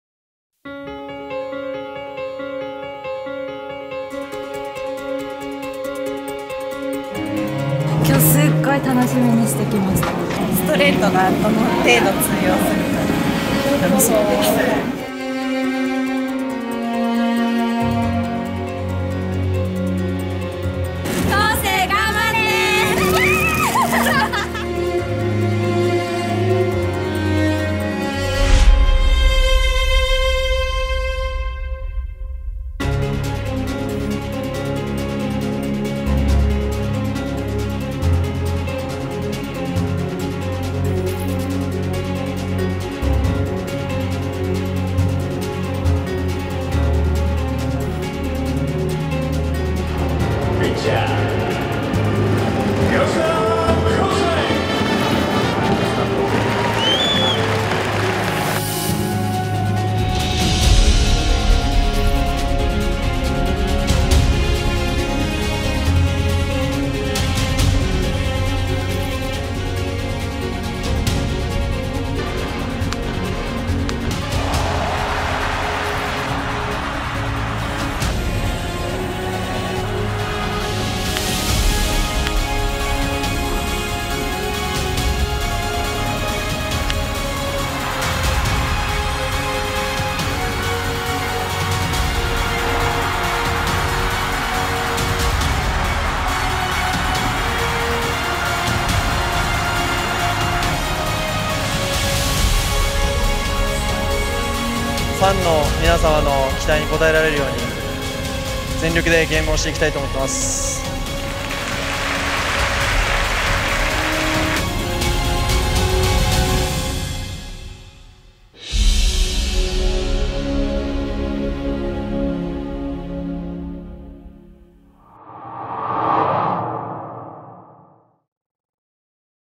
吉田輝星が輝いた日
注目のドラフト1位ルーキー吉田輝星投手が1軍デビュー戦で見事初白星を飾りました！ファンの方たちはその興奮をどう見ていたのでしょうか!?